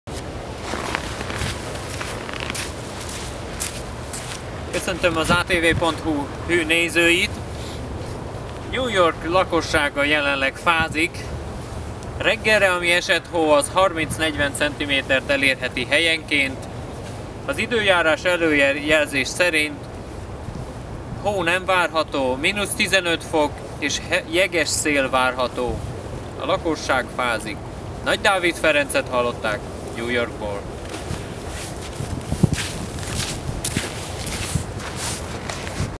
New York 2014 Snow Storm
Kemény fagy és hófúvások sújtják az Egyesült Államokat
 I support:
healthy-coffee.dxnnet.com [de], [hu], [en], [es], [pl],